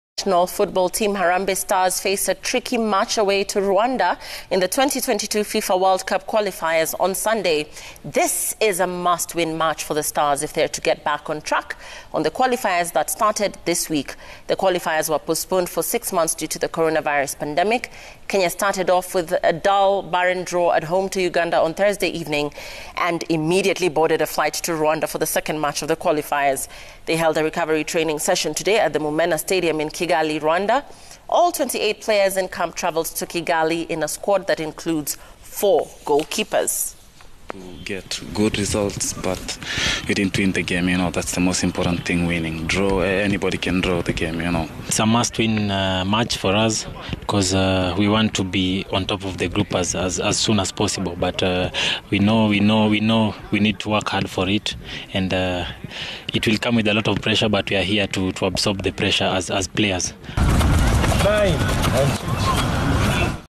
Harambee Stars arrive in Rwanda
Recovery session in Rwanda 24 hours after playing in Nairobi

Amavubi host Harambee Stars on Sunday in Kigali